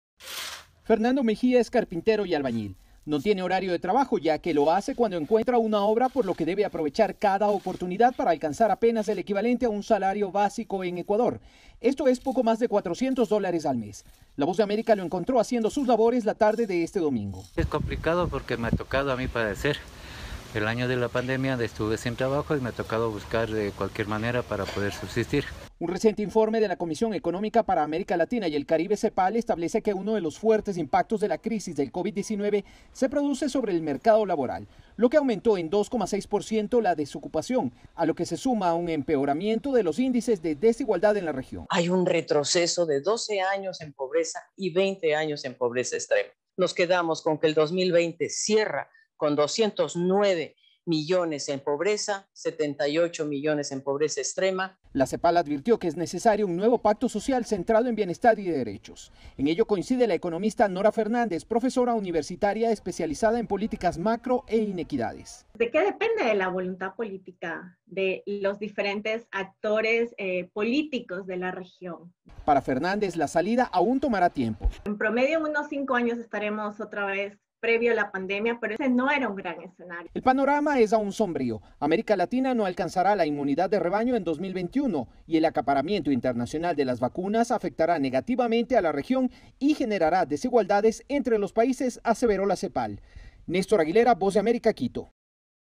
Preocupación tras informe "Panorama Social de América Latina 2020", publicado por la CEPAL
En América Latina se contabilizaron -al cerrar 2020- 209 millones de personas en condición de pobreza, 22 millones más que el año anterior. El dato se desprende de un reciente informe de la CEPAL. Desde Quito, Néstor Aguilera nos cuenta más.

¡Visítenos!